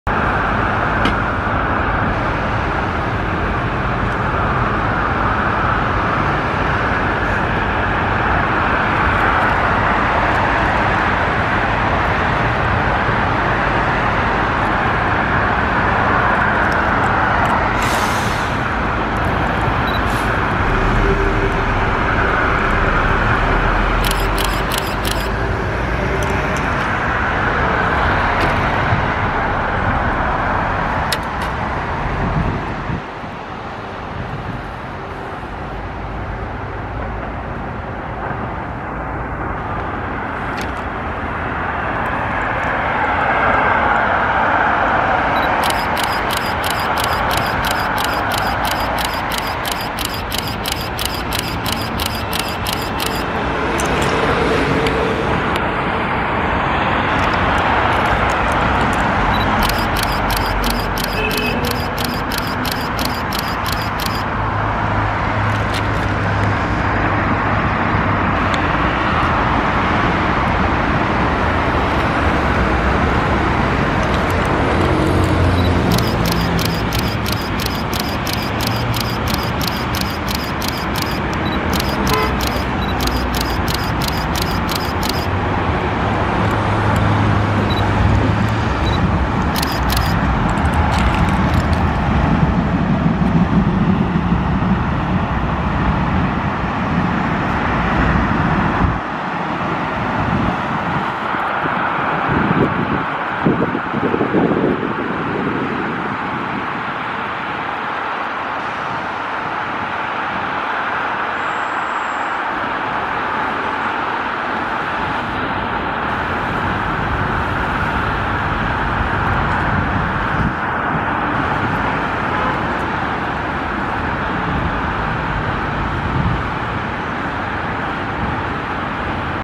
[4K 40x광학줌] 20230109 112807 헌 차(3X34) 줄께 새 차(3X35) 다오
새 차도 같은 번호였으면 제대로 였는데.. 한 끝 차이로..

촬영 : Canon PowerShot SX740 HS